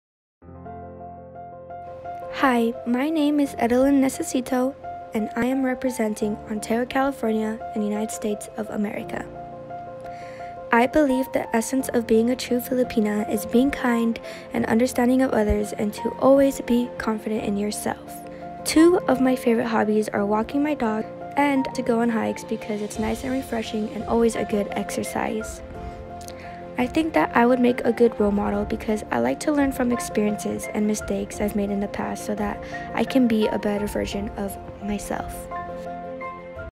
EDELYN NECESITO Candidate no. 15
EDELYN NECESITO
Candidate no. 15
Ontario, CA

She loves to make people laugh, even though she describes herself as being shy. She joined this pageant hoping to expand her self-confidence and get out of her comfort zone. She continues working on her studies to achieve her goal of becoming a licensed psychologist someday. mfi yourdestinyawaits.